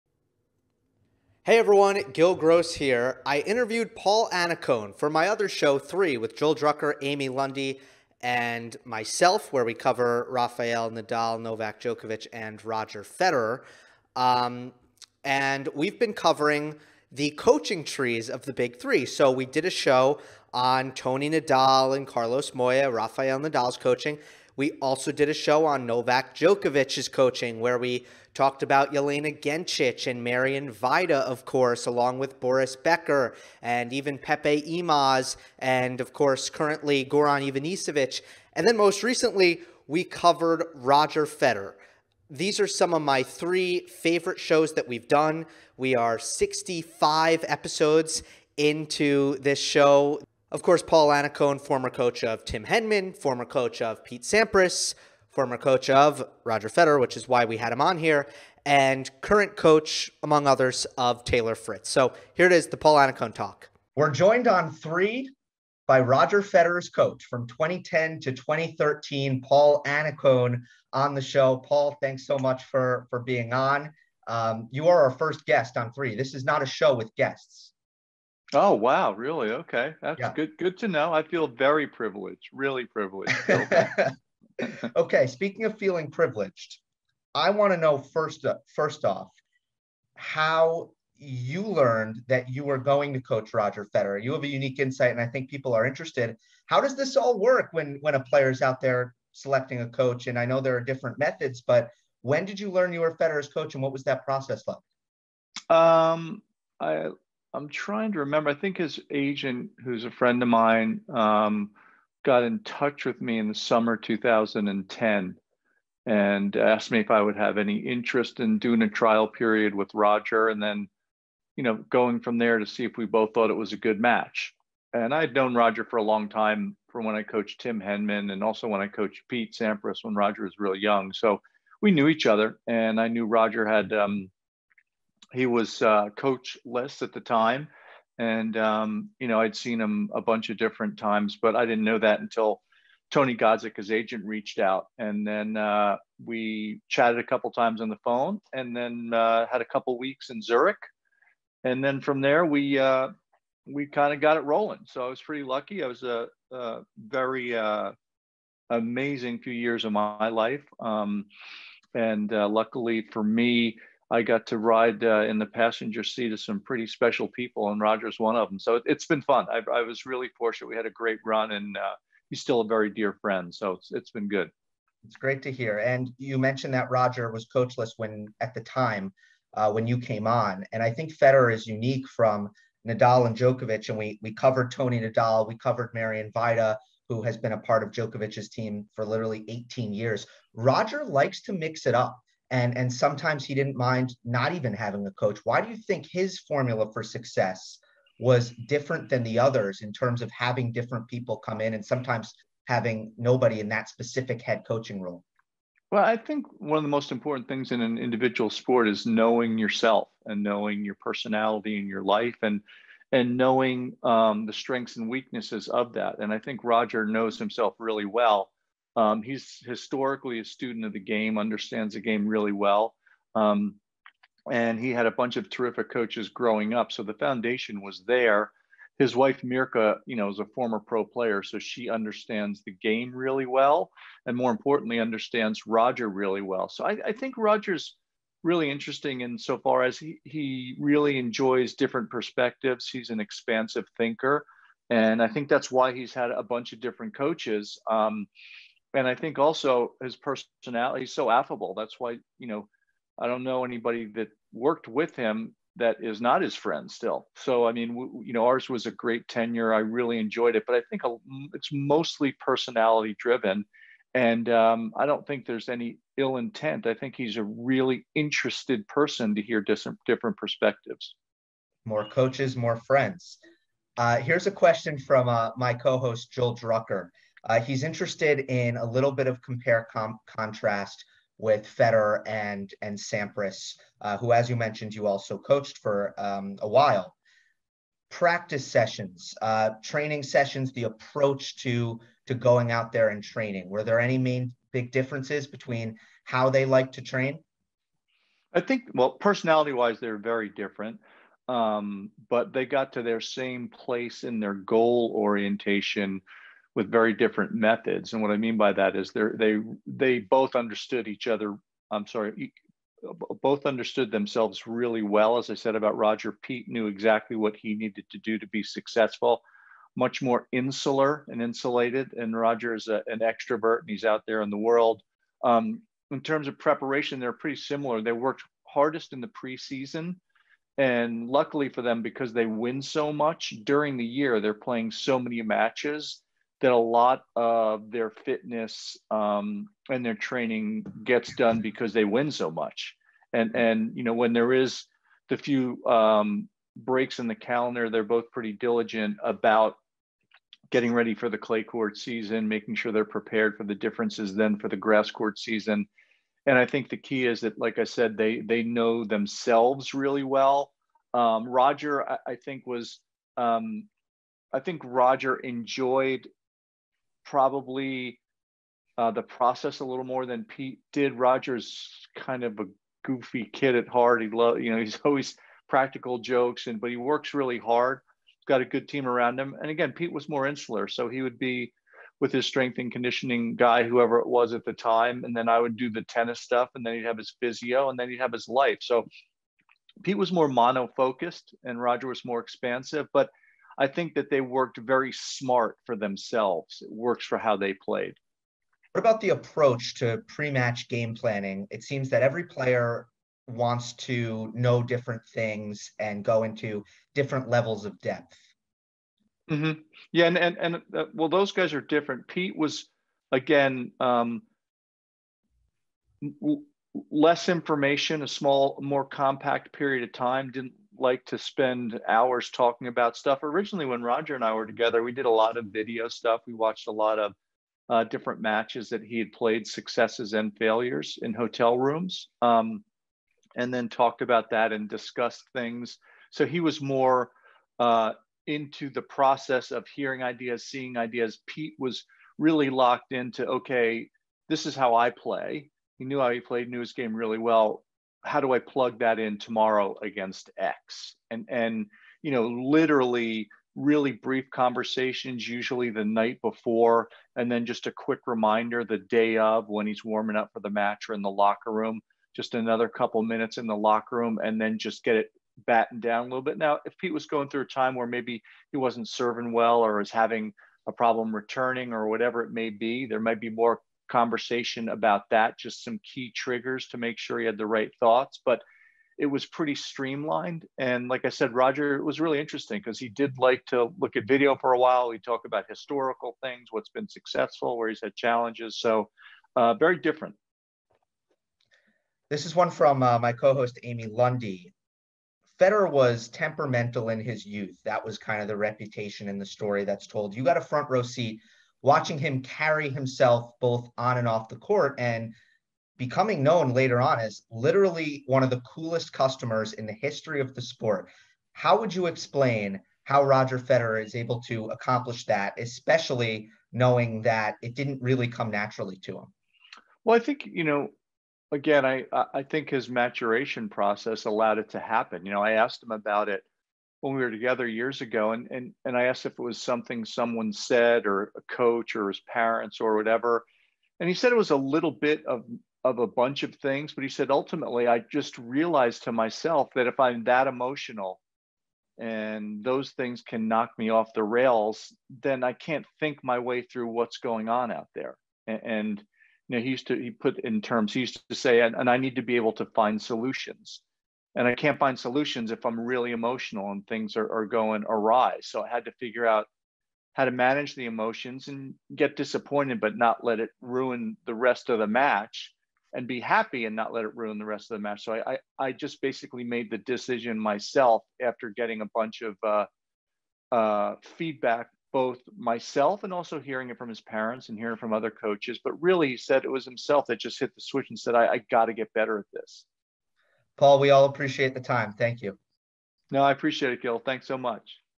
I interviewed Paul Annacone on Roger Federer
On Three --- A Tennis Show, we've been diving deep into the Big 3 coaching trees. After doing Rafael Nadal and Novak Djokovic, we decided to cover Roger Federer in part by speaking with his coach from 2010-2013, Paul Annacone. Annacone also coached Tim Henman and Pete Sampras, currently coaches Taylor Fritz and is a commentator o Tennis Channel.

Gill Gross provides in-depth ATP tennis analysis every Monday. Match breakdowns, the latest news, and other tennis insights...